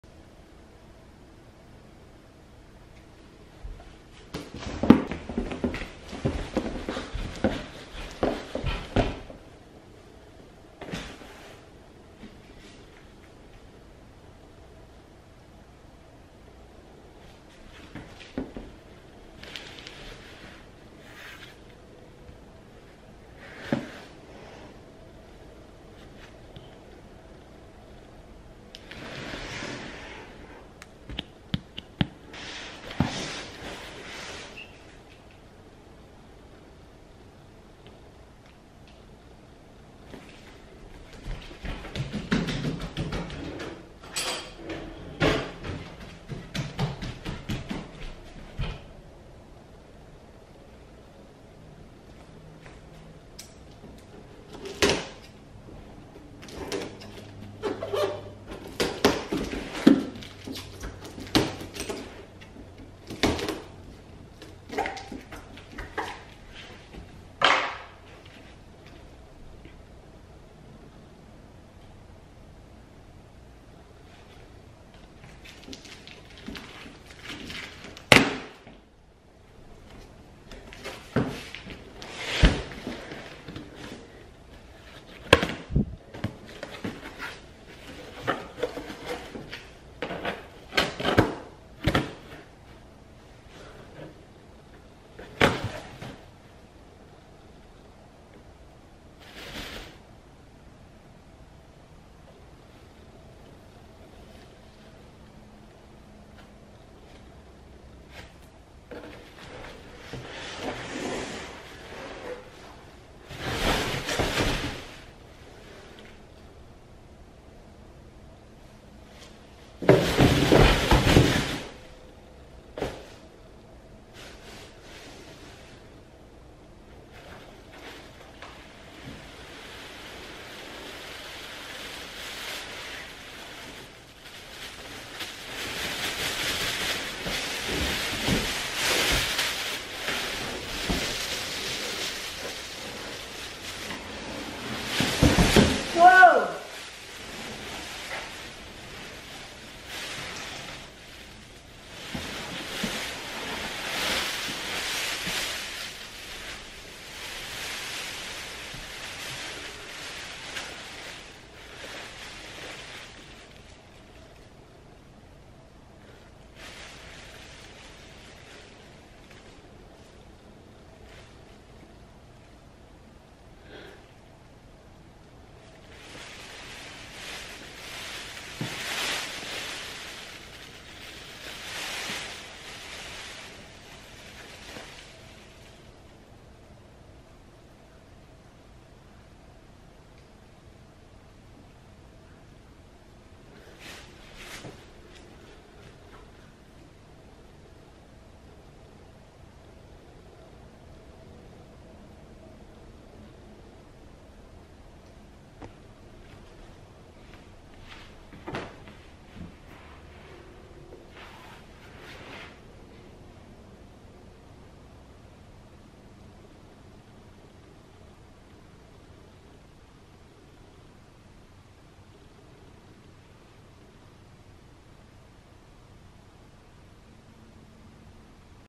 Alien Unboxing
Alien. Do Not Open.
homework for mime class